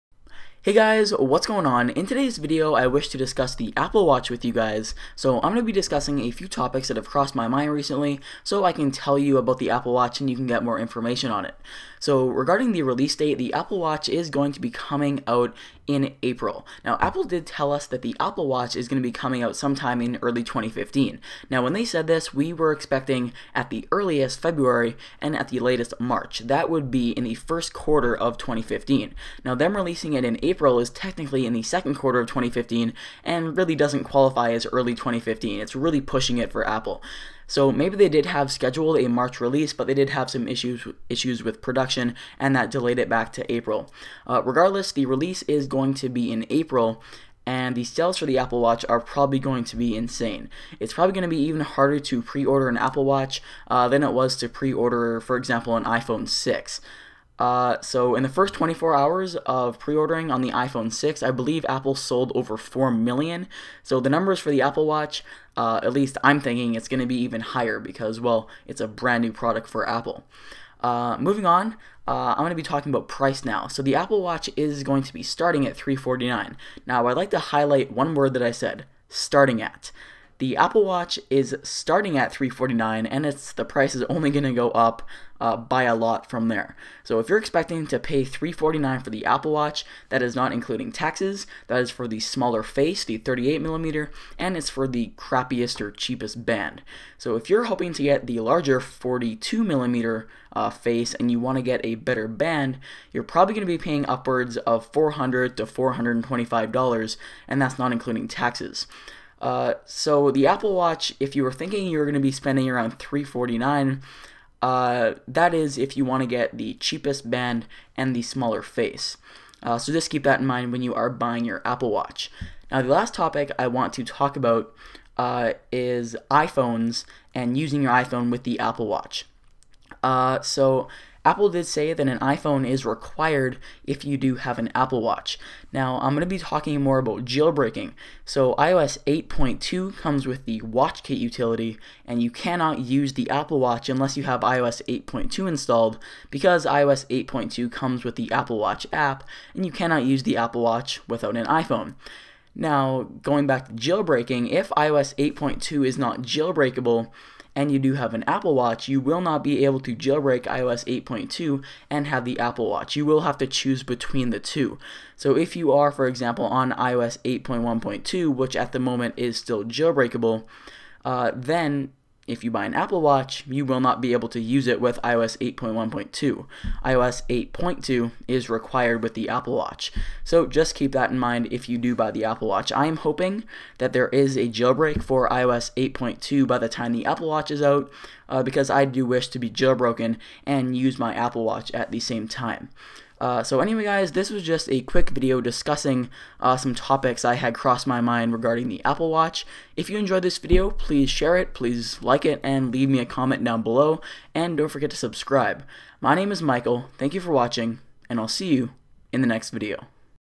Apple Watch Info - Everything You Need to Know!
Mike discusses the Apple Watch and everything you need to know about buying the watch in April.